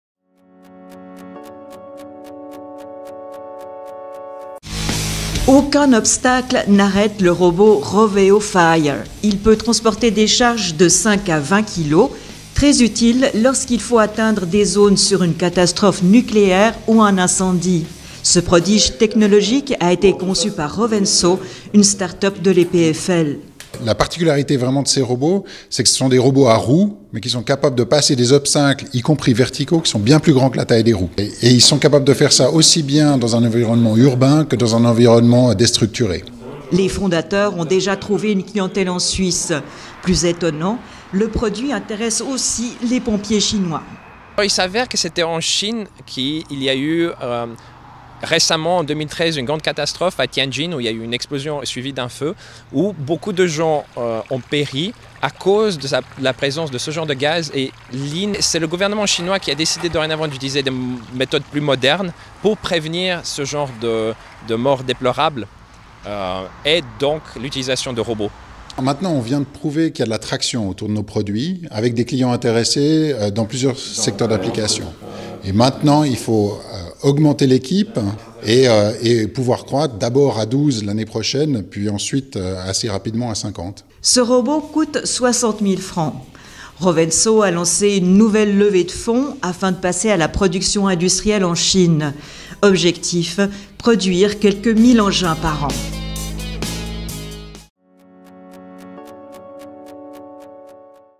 Rovenso, la start-up de l'EPFL qui séduit les pompiers chinois
Les trois cofondateurs de Rovenso ont développé un robot qu'aucun obstacle n'arrête. Capable de porter de lourdes charges, l'engin se révèle très utile pour atteindre des zones contaminées ou irradiées lors d'une catastrophe.
On dirait une sorte de tortue qui avance de manière butée. Equipées de quatre roues indépendantes, le robot terrestre ROVeo peut surmonter tous les obstacles. Développé par la start-up Rovenso issue de l'EPFL, l'engin a trouvé une clientèle en Suisse et aussi en Chine. Le concept a séduit les pompiers de ce pays et un prototype a été développé à Shenzen, mégapole industrielle de l'Empire du Milieu.

Les robots sont destinés aussi au marché du démantèlement des installations nucléaires ou pétrolières. Patron de Rovenso, Thomas Estier travaille avec deux cofondateurs: Lucian Cucu et Beat Geissmann. Cette jeune pousse soutenue notamment par la Fondation pour l'innovation technologique prépare une nouvelle ronde de financement. Objectif: devenir un leader mondial dans le domaines des robots terrestres civils.